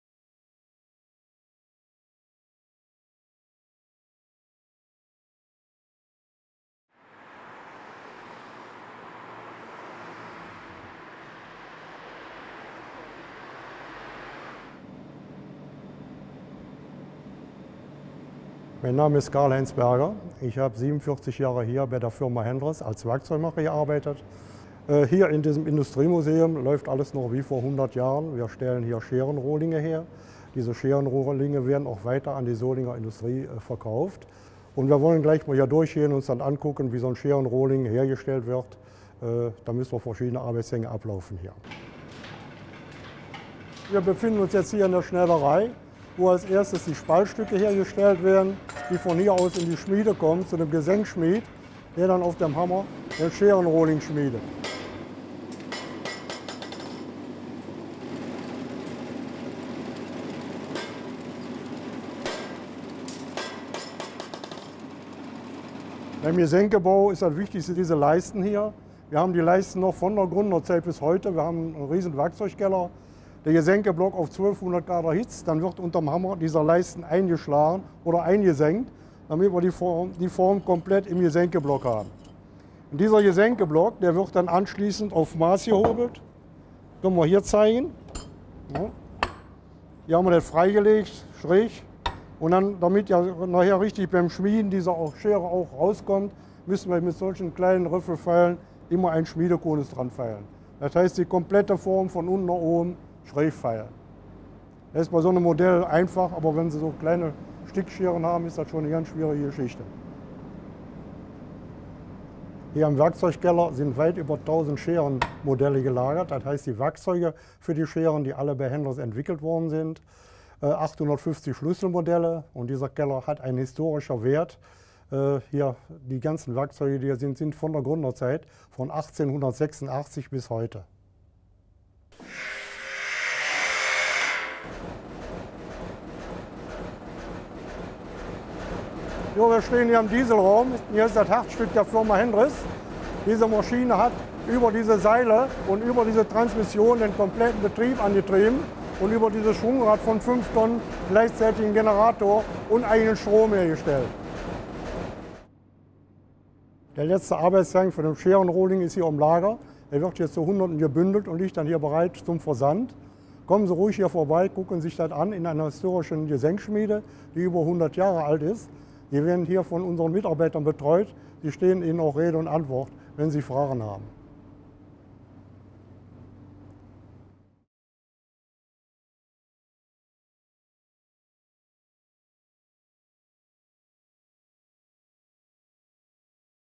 Gesenkschmiede Hendrichs
In der historischen Gesenkschmiede Hendrichs des LVR-Industriemuseums wird noch immer richtig produziert. Die Museumsschere wird vollständig in der Fabrik gefertigt. Noch heute erlebt man dabei, wie die rot glühenden Spaltstücke unter lautem Getöse zu Scherenrohlingen geschmiedet werden, wie es knallt und zischt -- und erfährt dabei Spannendes über die Geschichte der berühmten Solinger Schneidwarenindustrie.